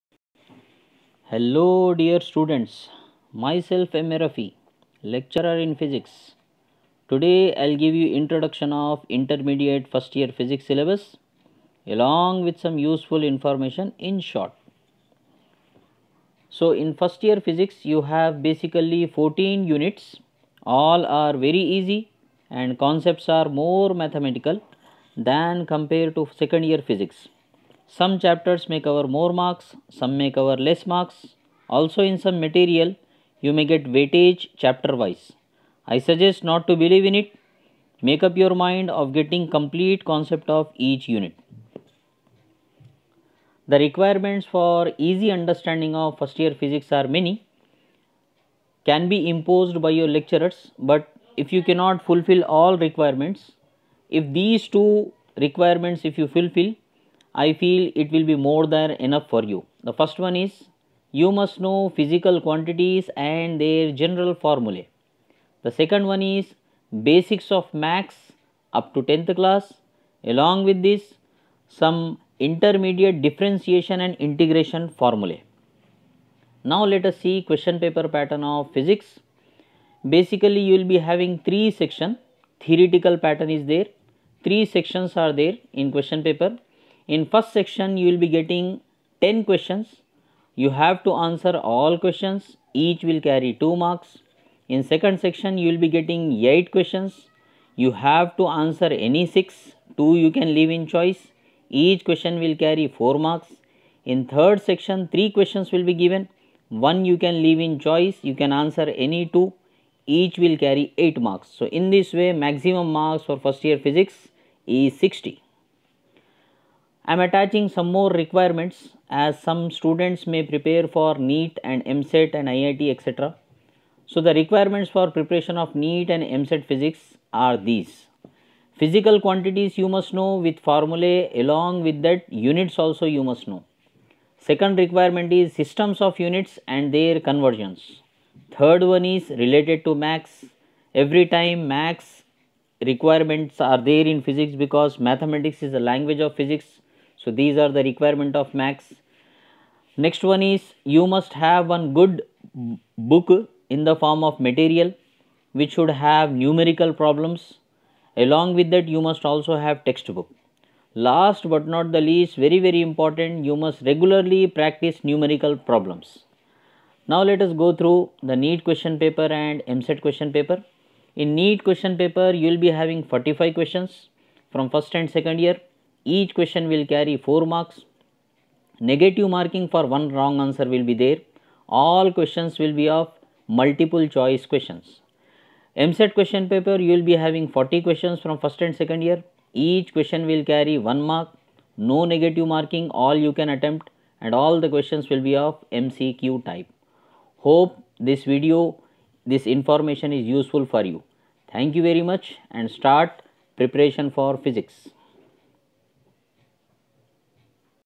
Inter First Year Physics introduction By Rafi Sir
Inter First Year Physics Syllabus , Model Paper , explanation about How to get prepare  NEET ,EAMCET  Physics